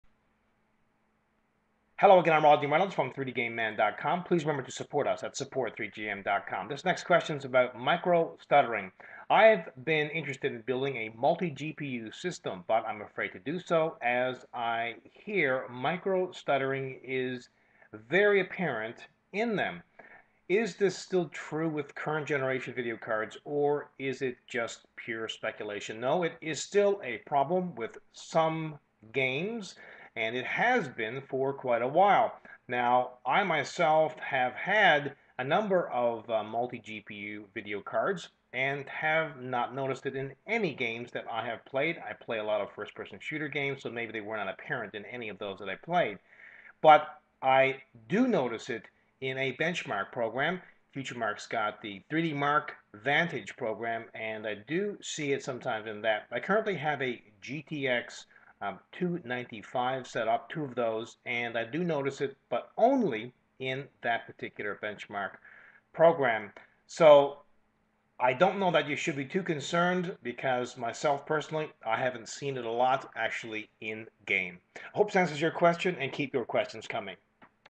#218 - Q&A: Micro Stuttering
Hi Rodney!

I have been interested in building a multi-GPU system (or possibly add a 4870x2 in my current system), but I'm afraid to do so, as I hear micro stuttering is very apparent in them.  Is this still true with current generation video cards, or is it just pure speculation?

Many Thanks,
oOoSmartiesoOo